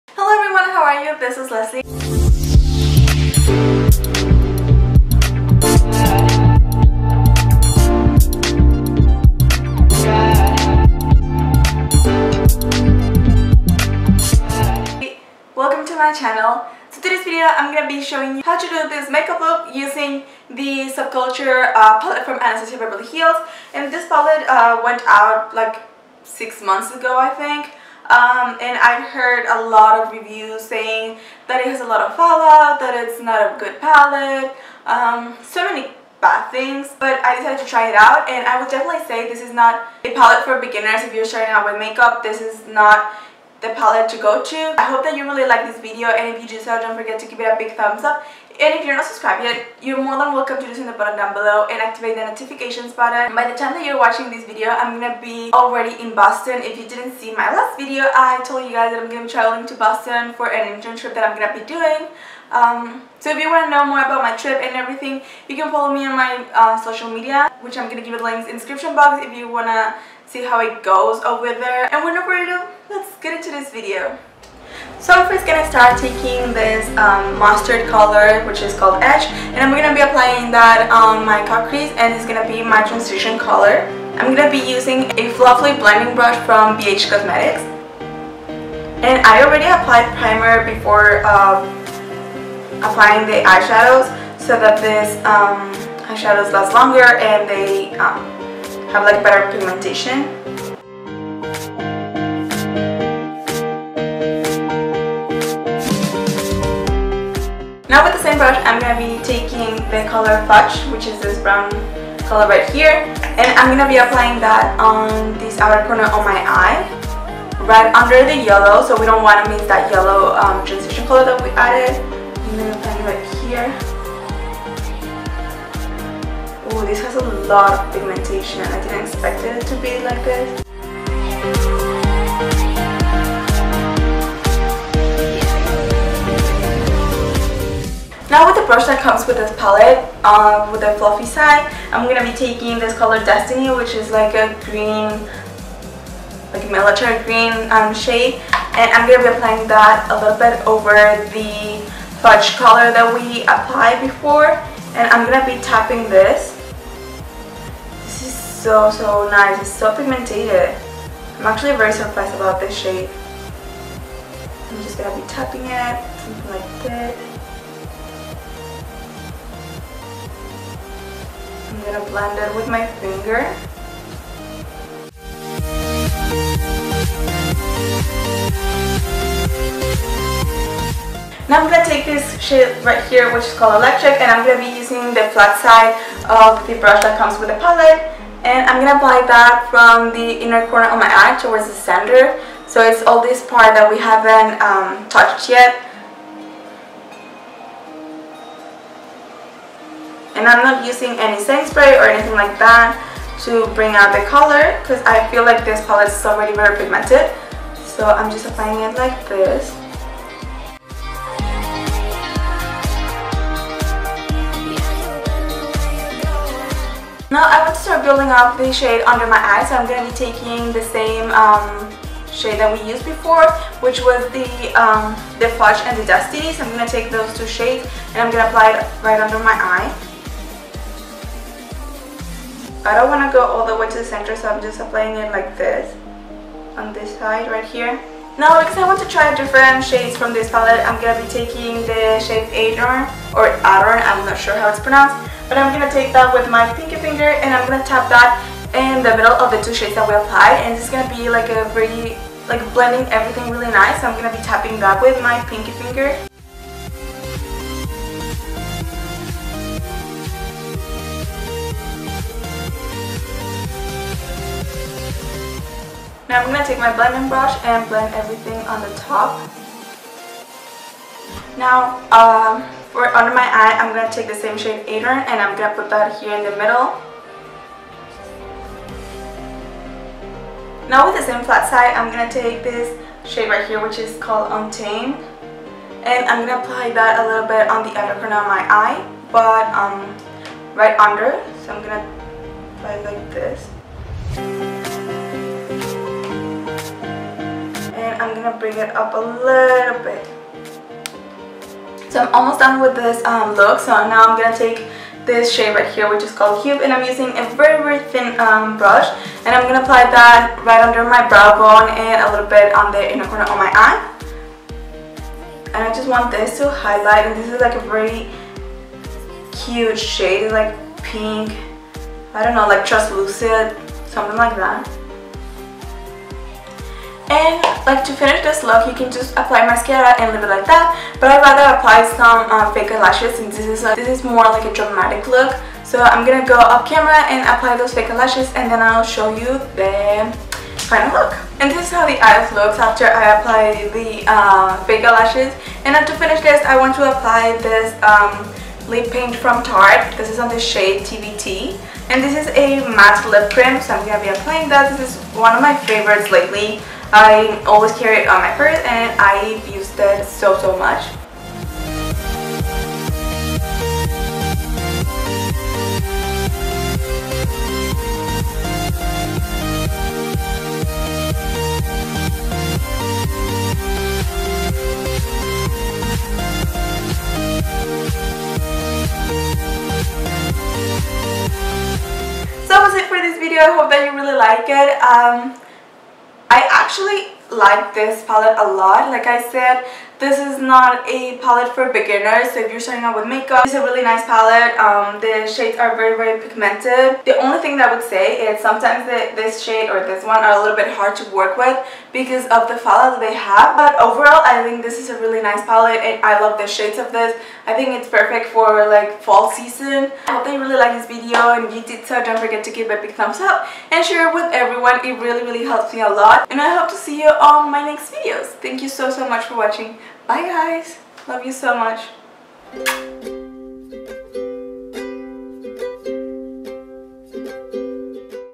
ABH Subculture Palette Tutorial + Review | Leslie Recendiz
Hi everyone!!
Even though this palette went out like 6 months ago, I wanted to give it a try and create a makeup look for you. It's definitely not a palette for beginners but I love the pigmentation!

♡ SOCIAL MEDIA ♡

Receive $10 on uber: leslier1860ue